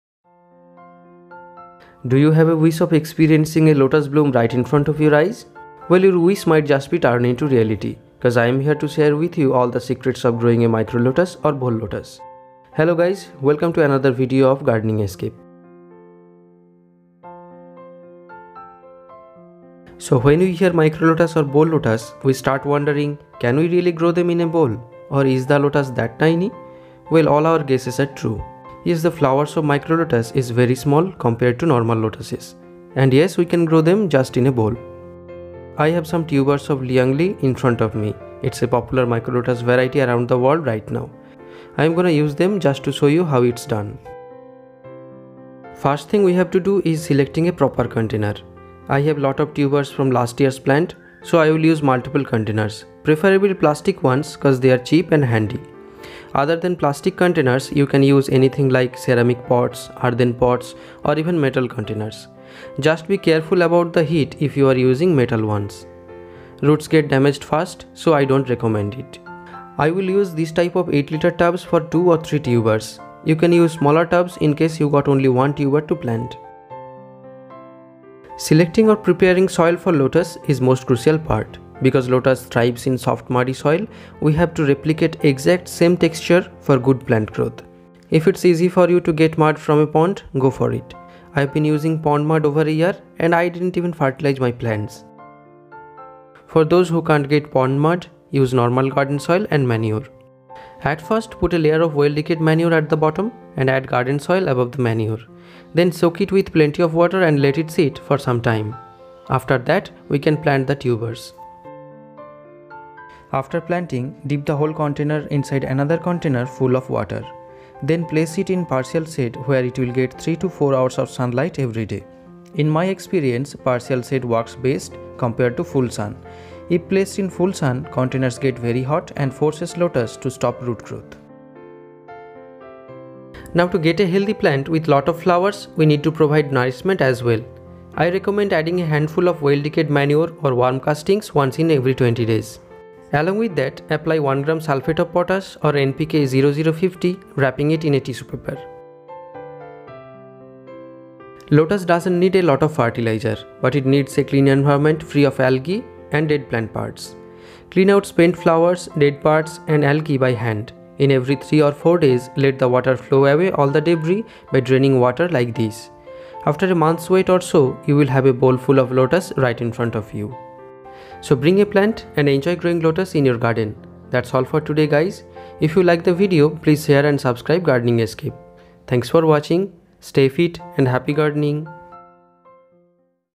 Growing Micro Lotus in a Container | How to Grow Lotus in a Bowl | Liang Li Lotus Care
Micro or bowl lotus is a dwarf cultivar of lotus family. It includes many popular names like Liang Li, Ameri camelia, Chinese micro red etc. Among them Liang Li grows really well in small container & has more petals compared to other micro lotuses. In this video I've discussed my way of growing lotus in a container. Hope you'll like it. 💚

• Join me @Gardening Escape  in these social media platforms...

• Links of other videos for You...